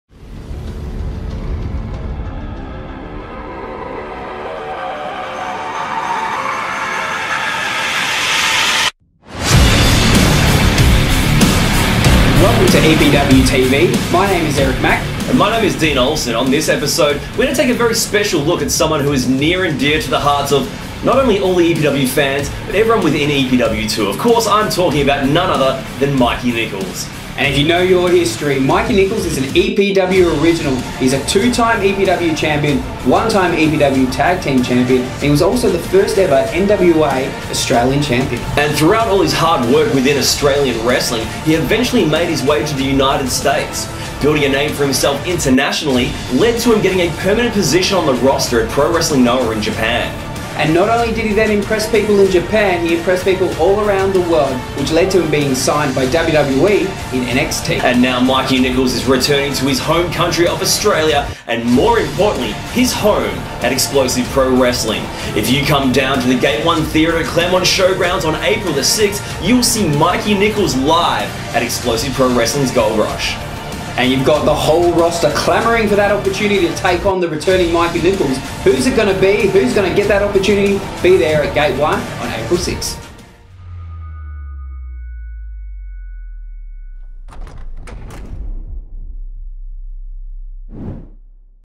EPWTV Episode Sixteen: Mikey Nicholls returns to EPW
It was a huge announcement last week that one of the best ever produced by Explosive Professional Wrestling was returning home, Mikey Nicholls.
He has wrestled for every major promotion around the world and has joined forces with CHAOS in NJPW. But before all of that he will be taking on an opponent at EPW's Goldrush on April 6th, as the locker room clambers for the opportunity we will have to stay tuned to find out who will be stepping up to the challenge.

If you want to check out any of our shows in full they are available at: